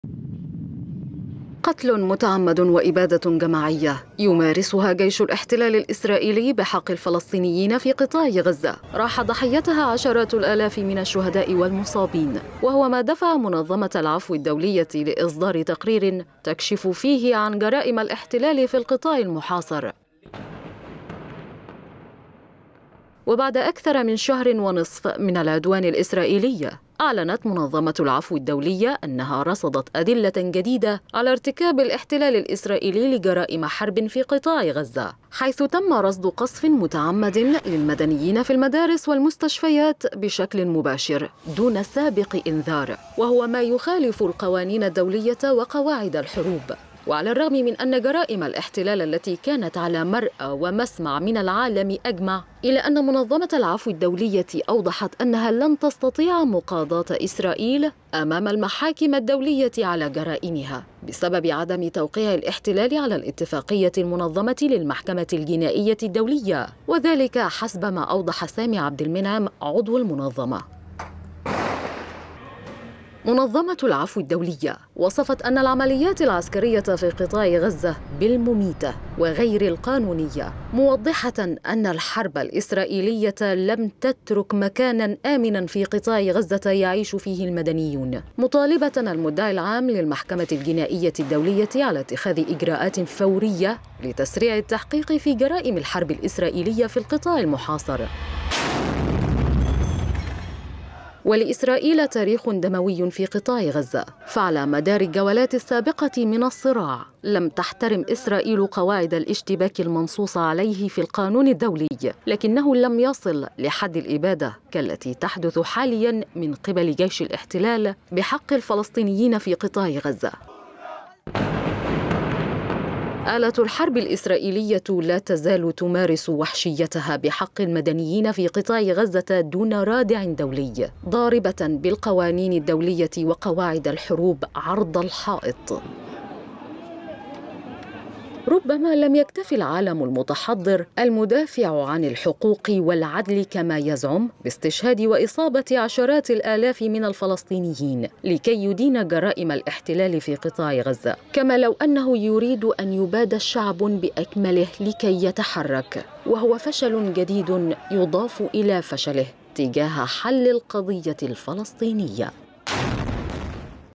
قتل متعمد وإبادة جماعية.. منظمة العفو الدولية تكشف جرائم الاحتلال
22/11/2023